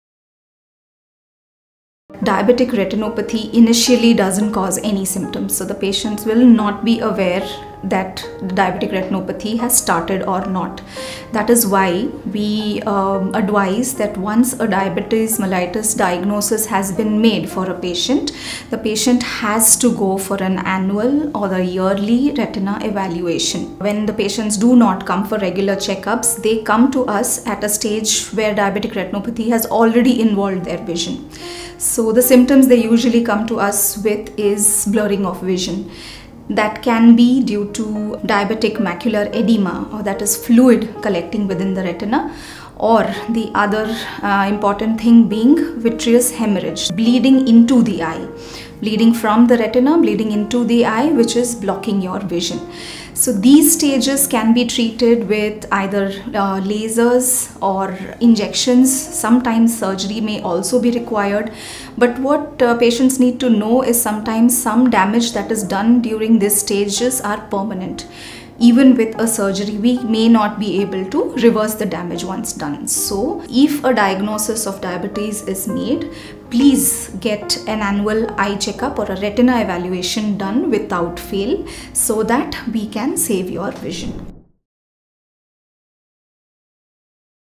How to Protect Your Eyes from Diabetic Retinopathy 👁️ | Dr. Rachita Explains
Worried about how diabetes can affect your vision? In this Short video, Dr. Rachita from Centre For Sight shares key tips on how to protect your eyes from diabetic retinopathy—a serious eye condition caused by uncontrolled blood sugar. From regular retina check-ups to managing your diabetes, blood pressure, and cholesterol, these expert insights can help you preserve your sight and catch issues early. Your vision matters—don’t ignore it!